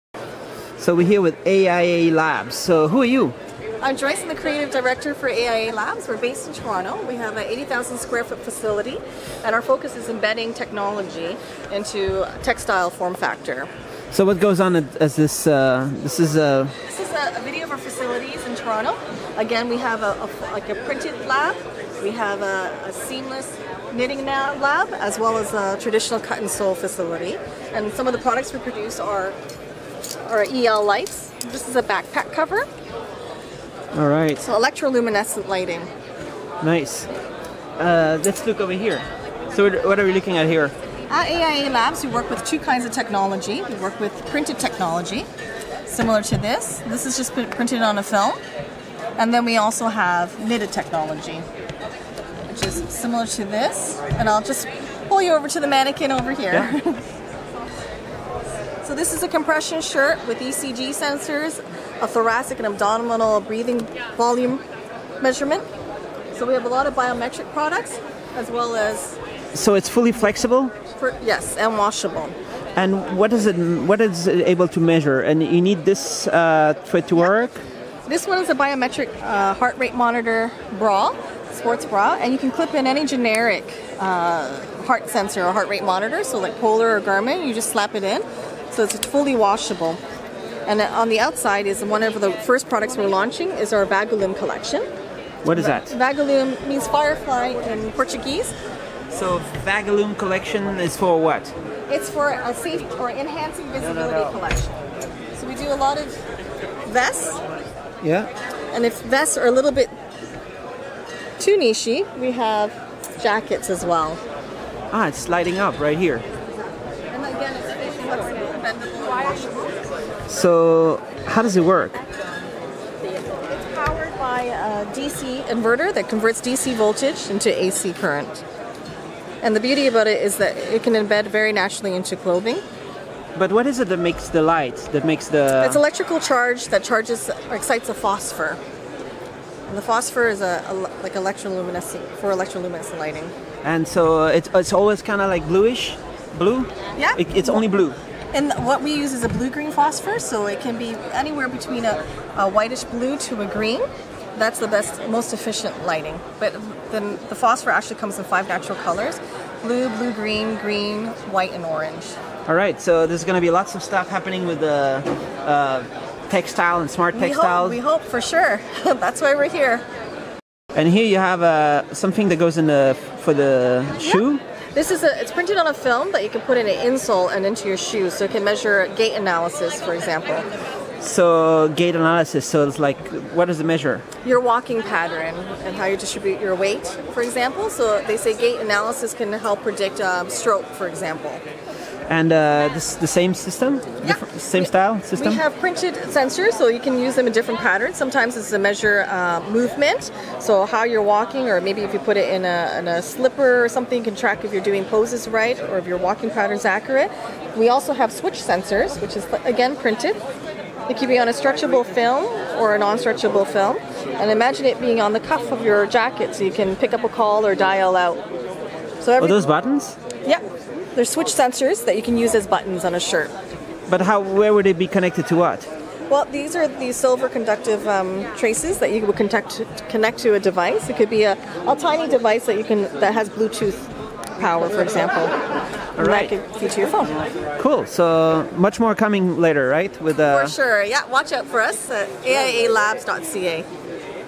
AIA Labs, Architects of Intelligent Applications (A.I.A.)
Interview with Architects of Intelligent Applications (A.I.A.) at IDTechEx Wearable USA. AIA designs, develops and manufactures wearable technology. Located in Toronto, Canada AIA’s facilities include a printed electronics laboratory, design studio, cut and sew floor and computerized knitting division.